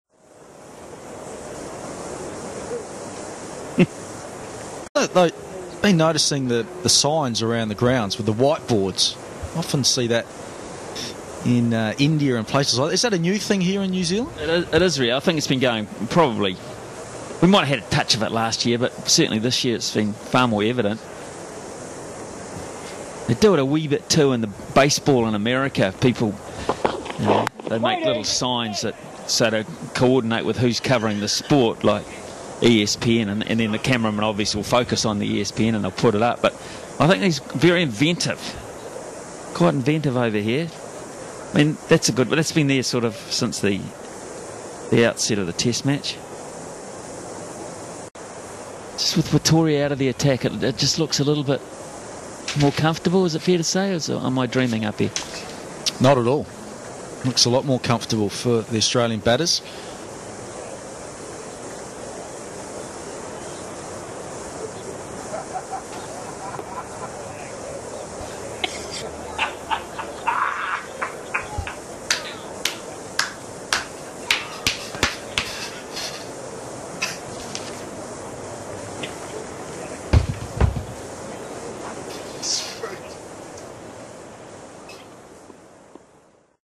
Scrotometer
2nd Test in Hamilton - discussing the comfort levels of the Aussie batsmen